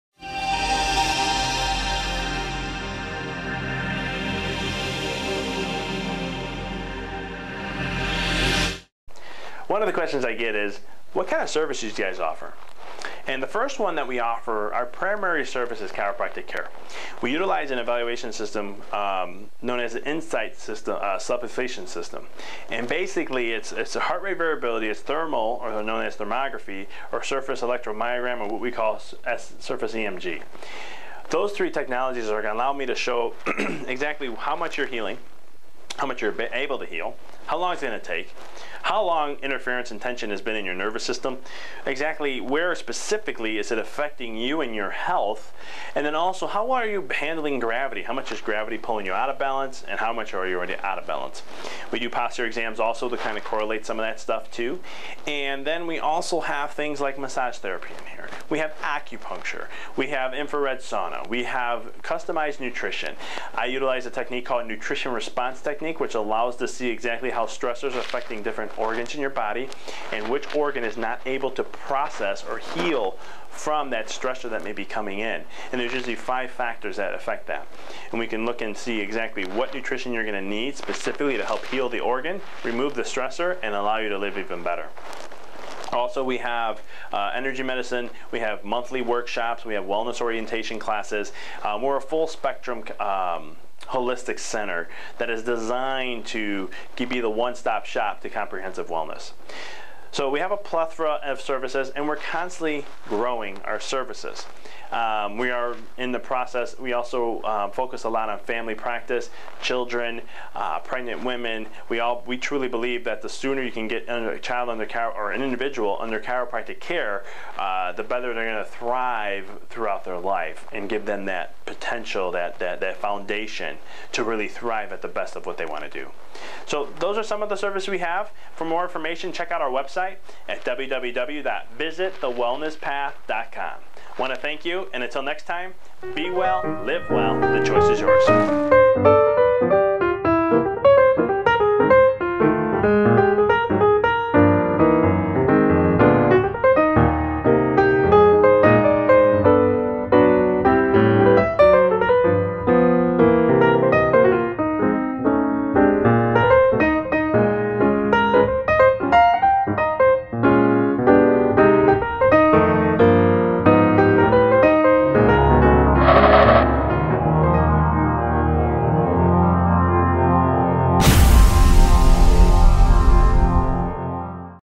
What Services Do We Offer?
At the Wellness Path, we have a laundry list of services that can help you and your family be at your best every single day.  From Chiropractic care, to Massage Therapy to Infrared Sauna, Weight Loss, Customized Nutrition and more.  We have the tools and systems to help you regain your health and help you back on your Wellness Path